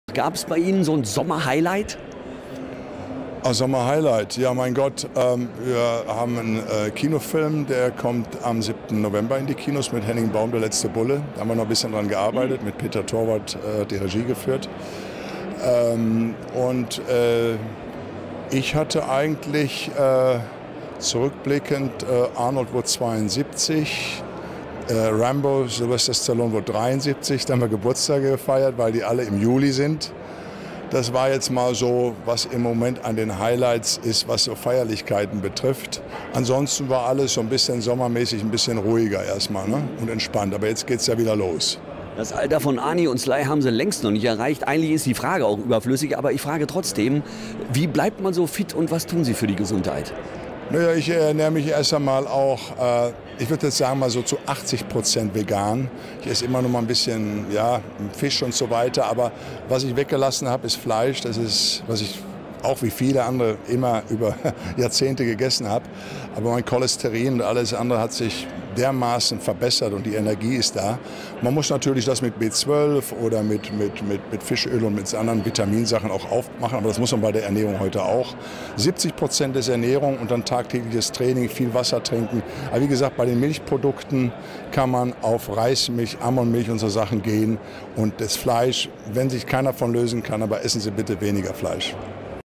Ralf Moellers Geheimnis: Super-fit mit 60!
Hollywoodstar Ralf Moeller plauderte mit uns in der Mercedes-Welt über seine Sommerhighlights 2019. Auch verriet er, wie er sich mit 60 Jahren fit hält!